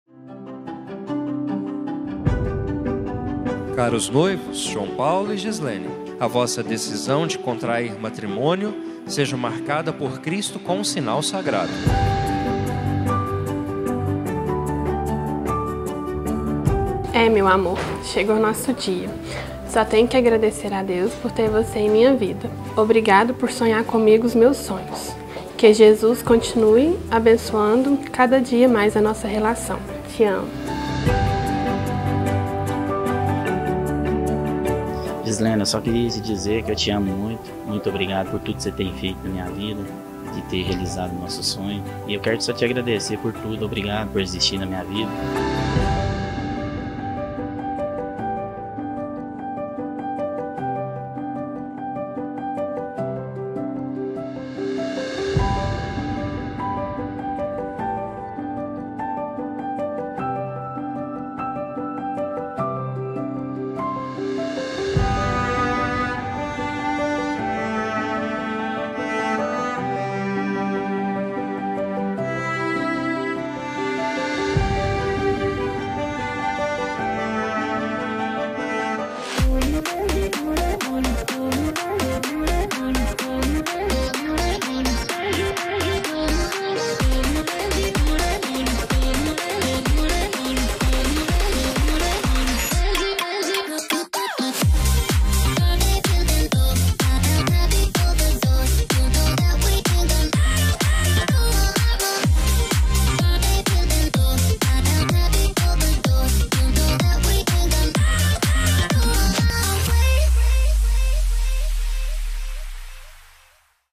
O casamento de Gislene e João Paulo
Mesmo depois de muitos anos juntos, o grande dia da Gislene e João Paulo foi super emocionante. A pequena Luna entrando com o João Paulo e ainda levou as alianças até ao altar. Quer ver um pouco mais?